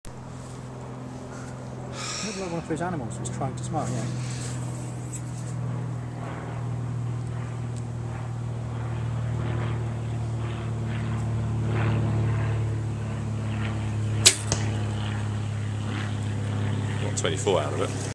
Owen's Compound Face!
Lol - Owen and his interesting facial expressions during the draw :)